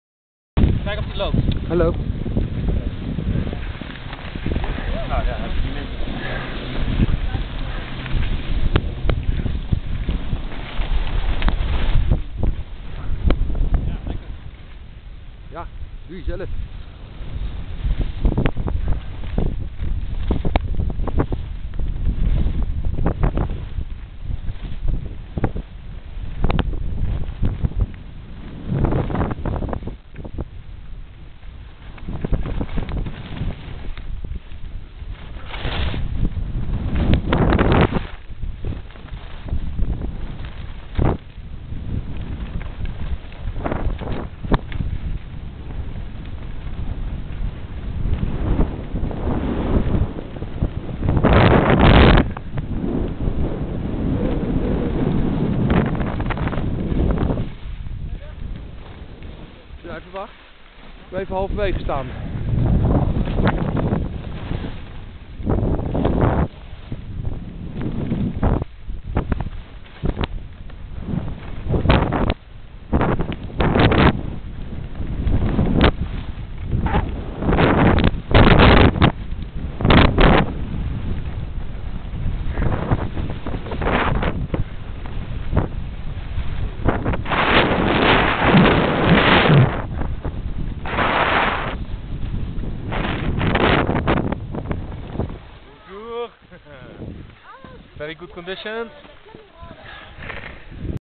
Afdaling snowboard Vaujany Les Grandes Rousses 2008
Afdaling op halve snelheid gefilmd door een trage jeroenski